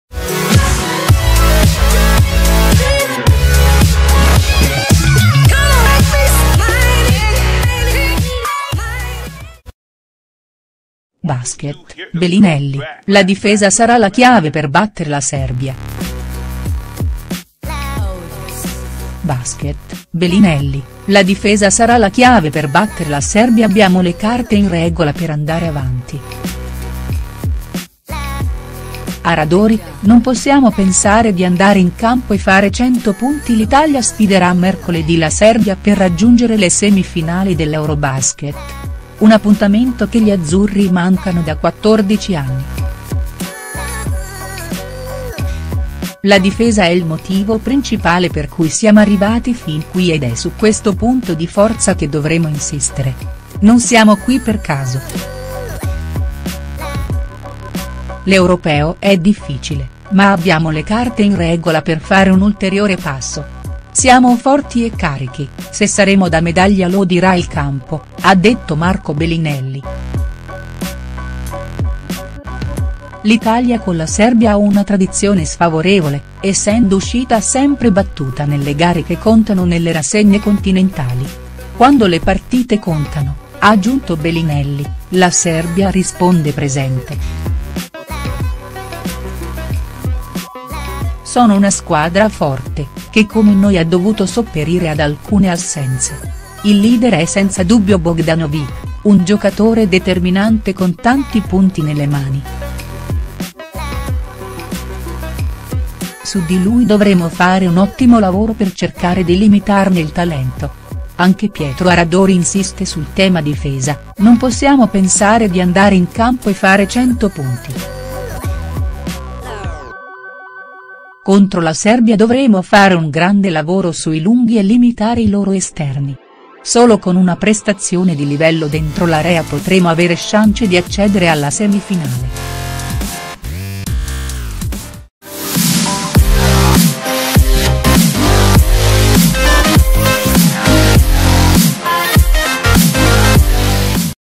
Basket, Belinelli: "La difesa sarà la chiave per battere la Serbia".HD
"Abbiamo le carte in regola per andare avanti". Aradori: "Non possiamo pensare di andare in campo e fare cento punti"